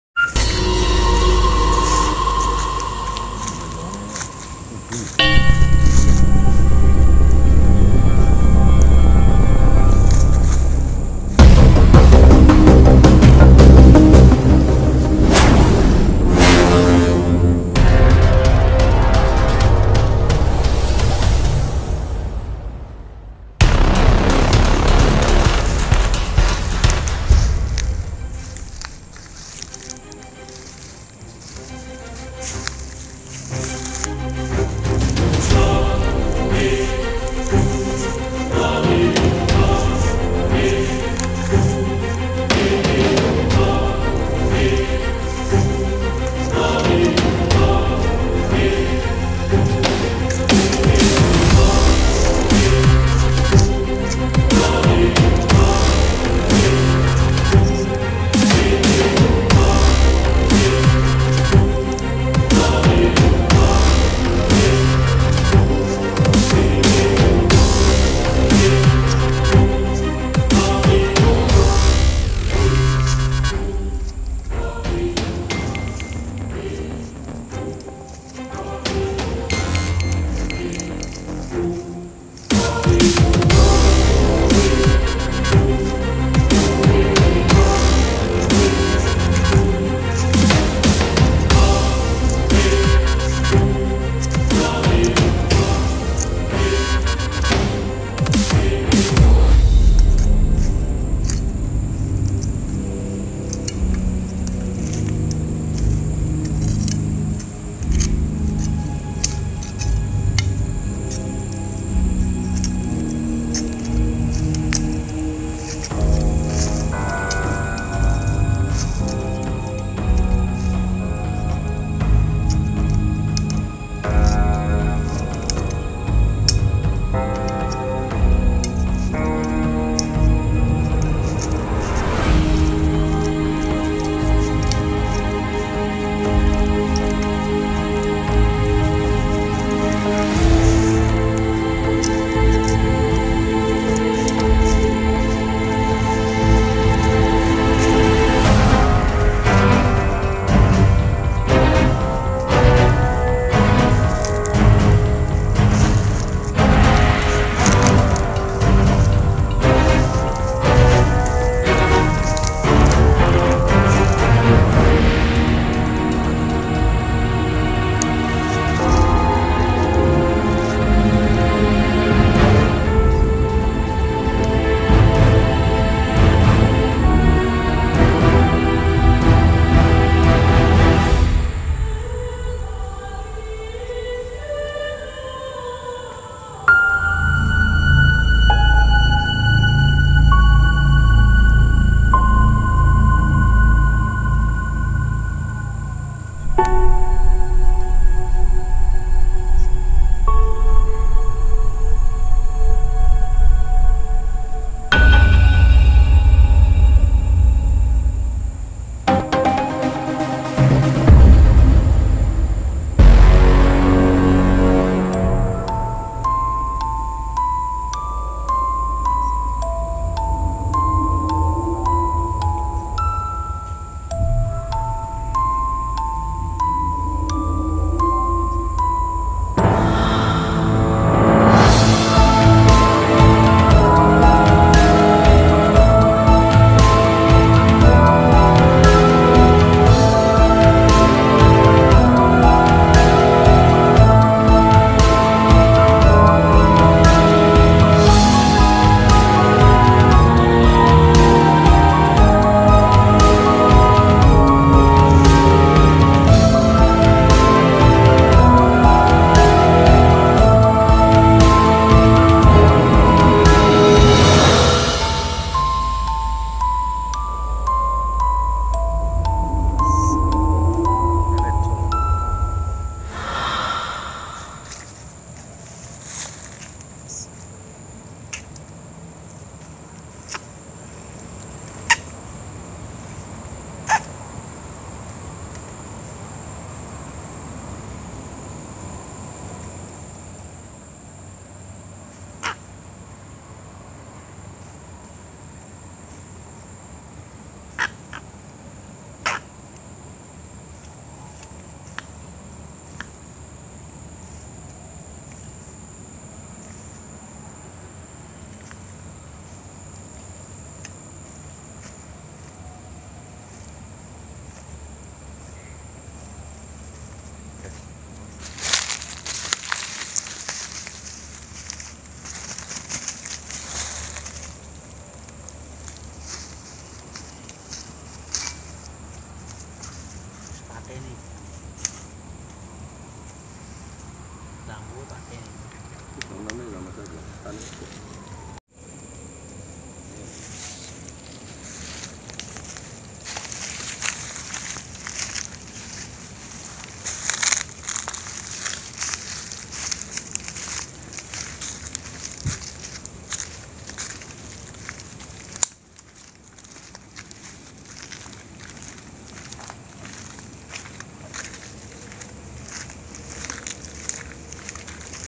TATA CARA RITUAL PENAKLUKKAN KHODAM BAMBU PETUK | Belantara Media
Ini adalah prosesi penaklukkan penghuni atau khodam bambu petuk (pethuk) di alam liar. Proses ini dilakukan oleh praktisi spiritual, jangan coba-coba meniru tanpa didampingi ahlinya. Belantara Media berkesempatan mendokumentasikan ritual singkat ini untuk sekedar berbagi pengalaman. Selamat menyaksikan..

Bambu Petuk Asli
Bambu Pethuk
Mencari Bambu Petuk Di Alam
cara mencari bambu petuk
ritual
David Feridian
Paul Hendrawan
Majalengka
Cirebon